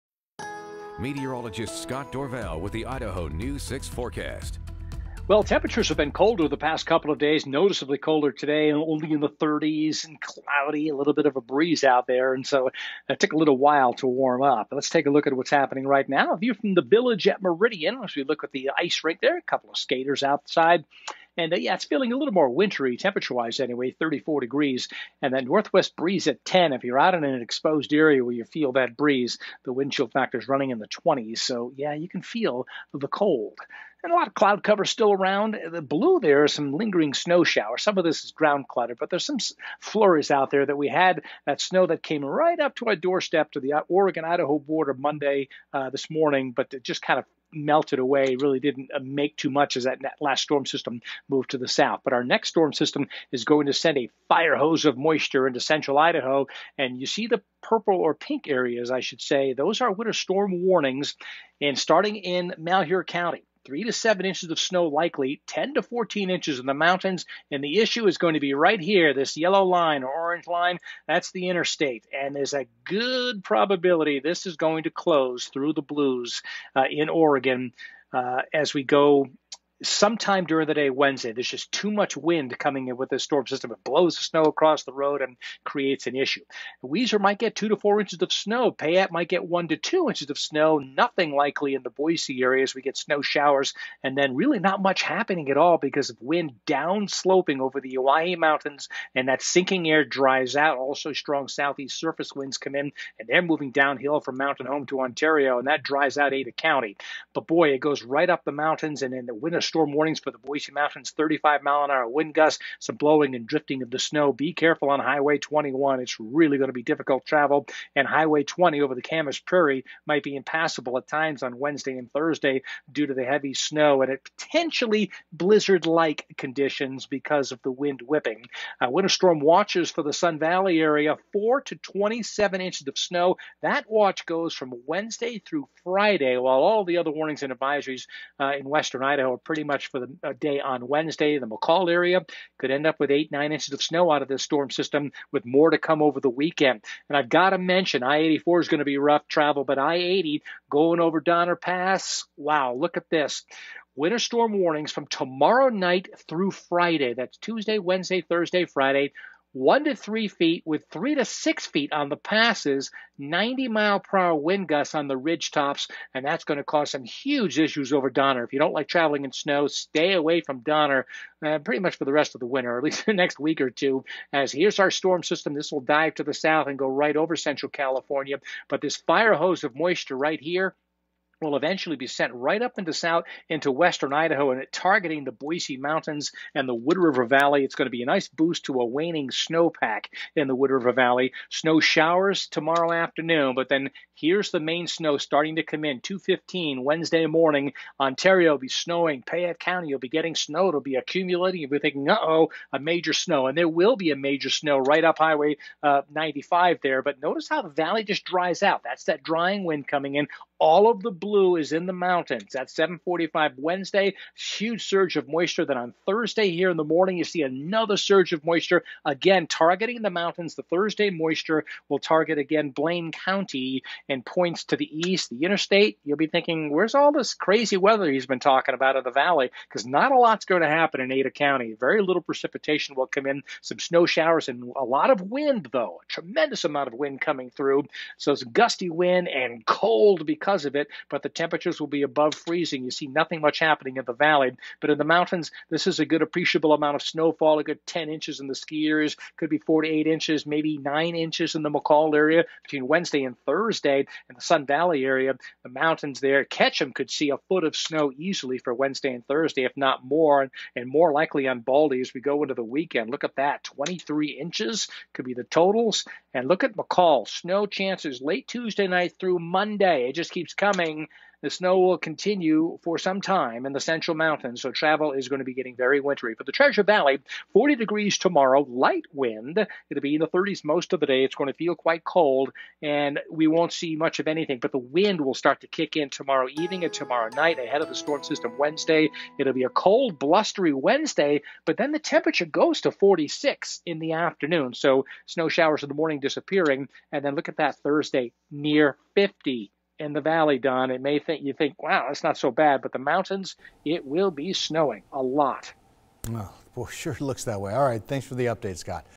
Scott Dorval's Idaho News 6 Forecast - Monday 1/25/21
Continued Cold Tuesday with Some Sunshine